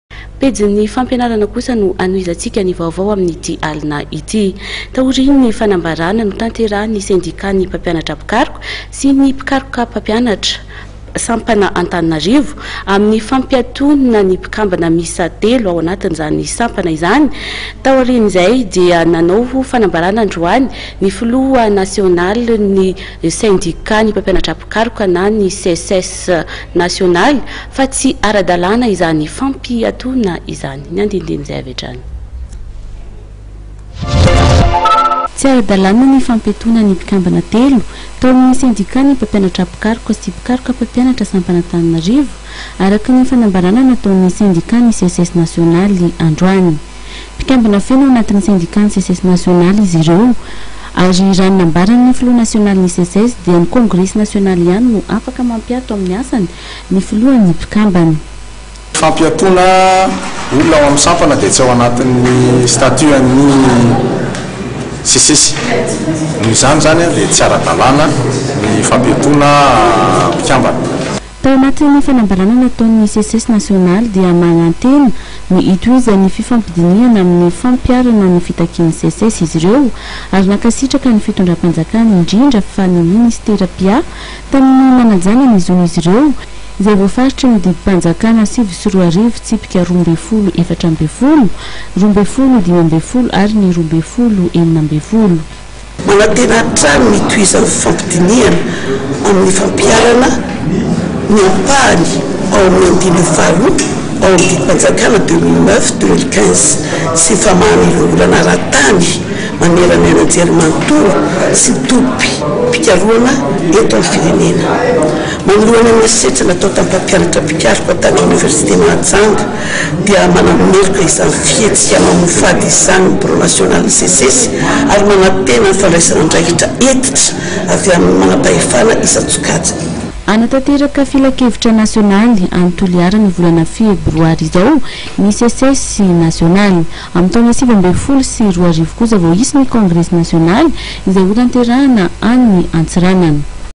03 SECES
Taorian'ny fanambaran'ny sendikan'ny mpampianatra mpikaroka sy mpikaroka mpampianatra seksiona Antananarivo amin'ny fampiatoana ny mpikambana telo amin'ny sampana Antananarivo izay ao anatin'izay ny filoha nasionly ny SEcES dia nanambara ity farany fa tsy ara-dalàna izany fampiatona izany.